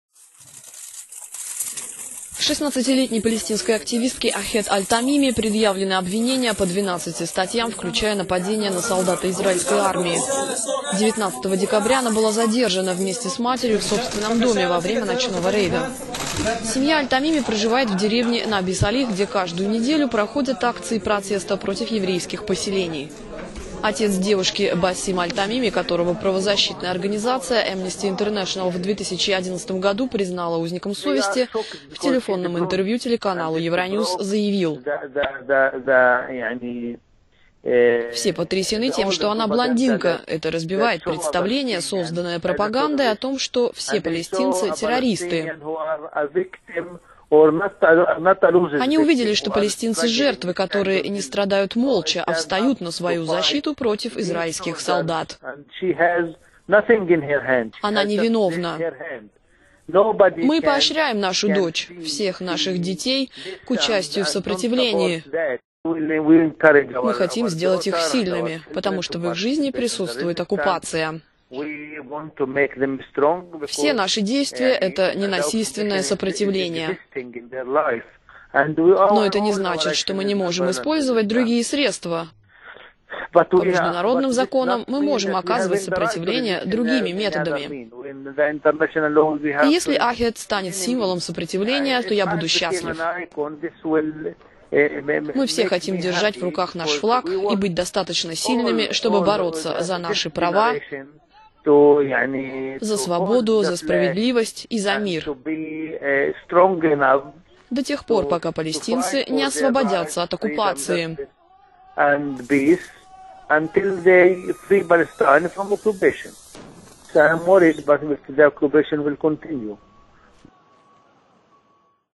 "Палестинцы не страдают молча"
Интервью с отцом задержанной палестинской активистки Ахед аль-Тамими…